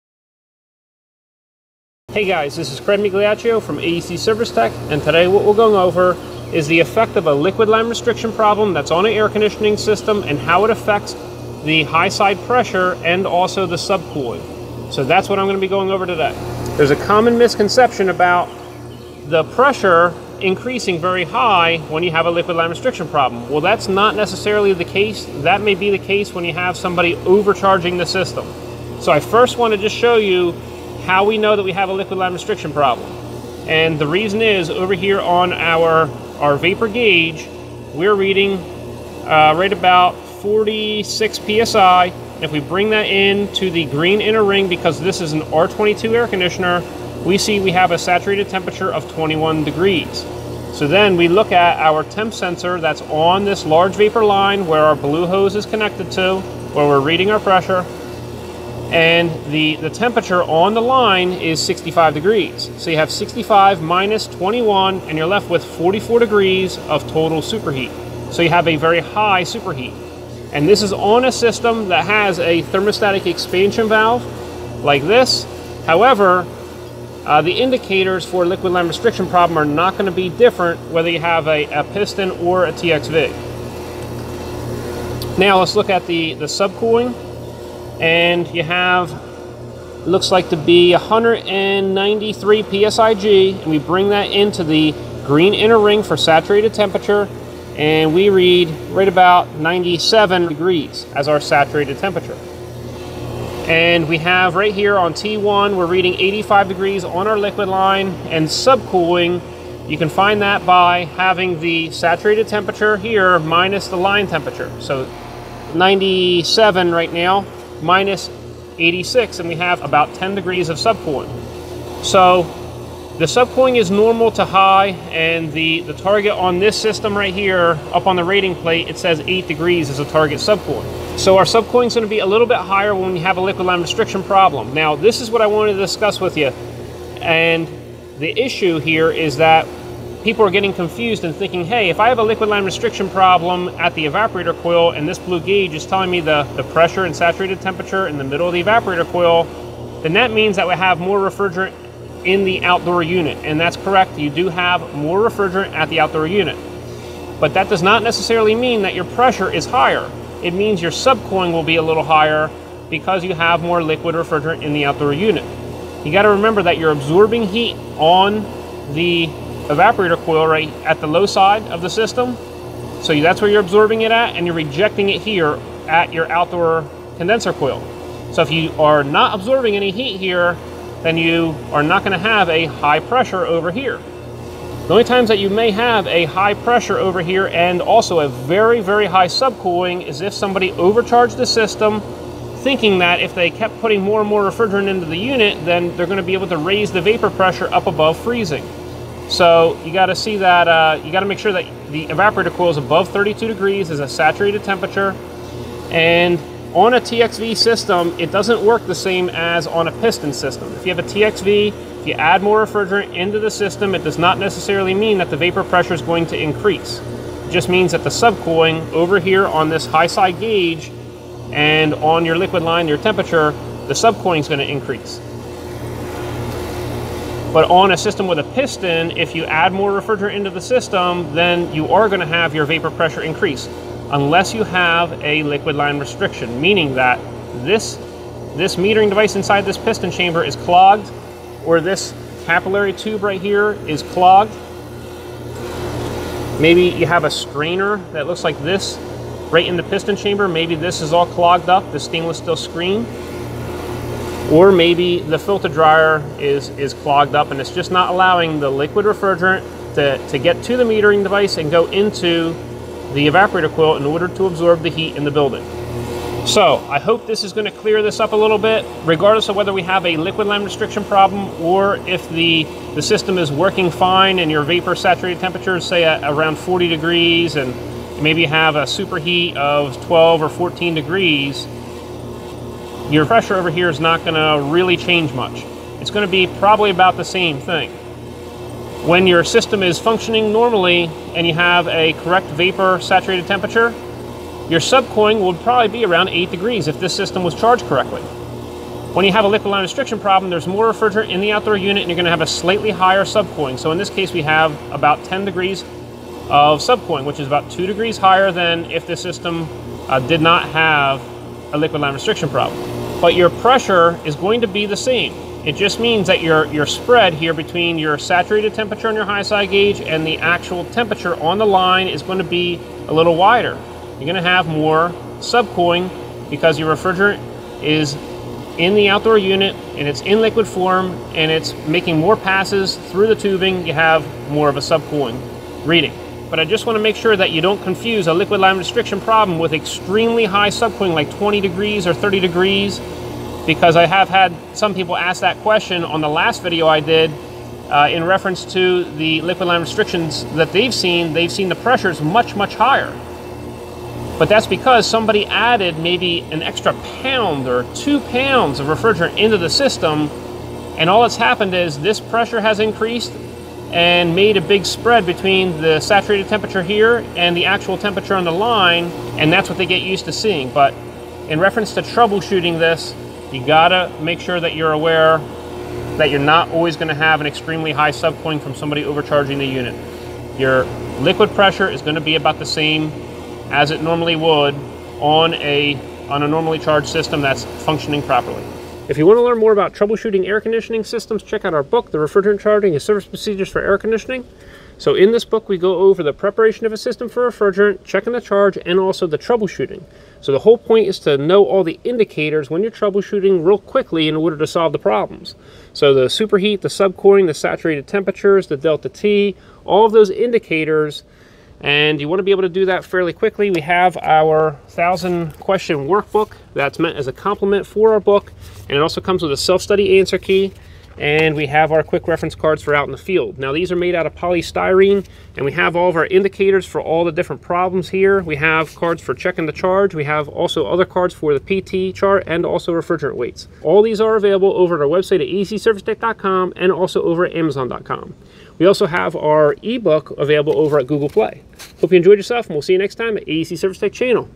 Liquid Line Restriction! What Happens to High Side Pressure & Subcooling?
In this video, I show how a Liquid Line Restriction Affects The Liquid Pressure and Subcooling. I go over the indicators of a Restriction such as vapor saturated temp, superheat, subcooling, & Delta T. A restriction could be caused by a clogged strainer, clogged metering device, a TXV with low bulb pressure, or a filter drier. Supervision is needed by a licensed HVACR Tech while performing tasks as Experience and Apprenticeship garners Wisdom and Safety.

Yellow Jacket Refrigerant Gauge Set
Refrigerant hoses with valves
Fieldpiece ST4 Dual Temp Meter